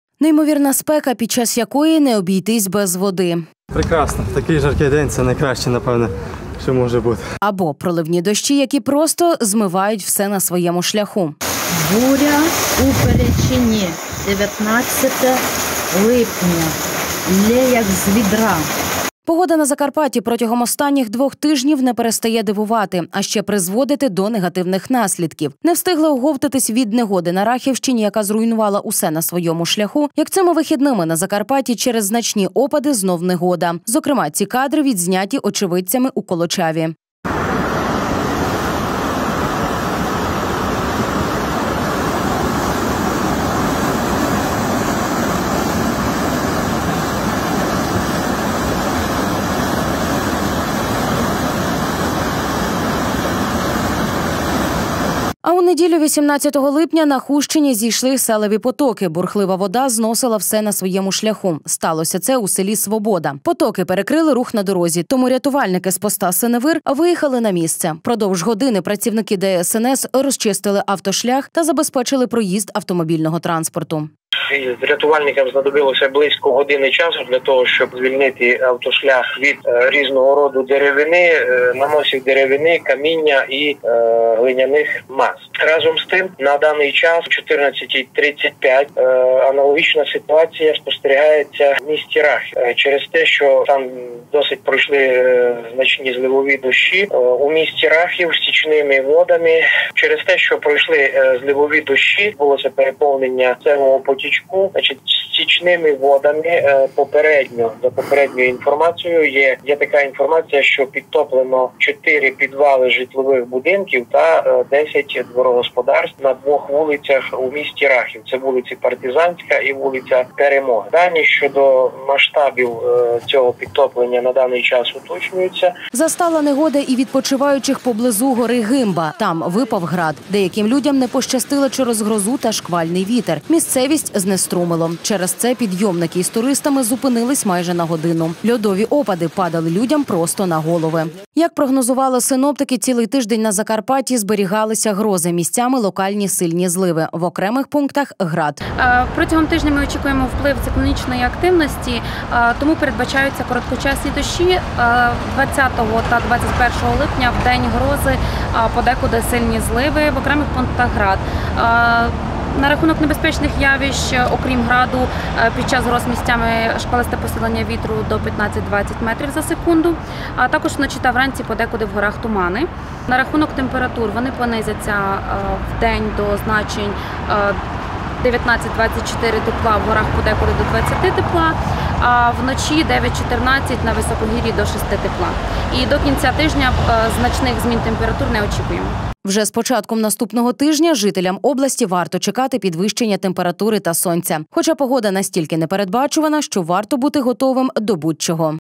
Наслідки негоди. Чого чекати далі?
Неймовірна спека. Під час якої не обійтися без води. Або приливні дощі, які просто змивають все на своєму шляху. Погода на Закарпатті протягом останніх 2-х тижнів не перестає дивувати. А ще призводити до негативних наслідків, не встигли оговтатись від негоди на Рахівщині, яка руйнувала усе на своєму шлях, як цими вихідними на Закарпатті через значні опади  знов негода. Зокрема ці кадри відзняті очевидцями у Колочаві. Ну, а у неділю 18 липня на Хустщині зійшли селеві потоки, бурхлива вода зносила все на своєму шляху, сталося це у селі Свобода. Потоки перекрили рух на дорозі, тому рятувальники з поста Синевир виїхали на місце. Впродовж години працівники ДСНС розчистили автошлях та забезпечили проїзд автомобільного транспорту. Застала негода і відпочиваючих поблизу гори Гимба, там випав град, деяким людям не пощастило через грозу та шквальний вітер місцевість знеструмило, через це підйомники із туристами зупинились майже на годину, льодові опади падали людям просто на голови. Як прогнозували синоптики цілий тиждень на Закарпатті зберігалися  грози, місцями локальні сильні зливи, в окремих пунктах град. Вже з початком наступного тижня, жителям області варто чекати підвищення температури, сонечко. Хоча погода настільки непередбачувана, що варто бути готовим до будь чого.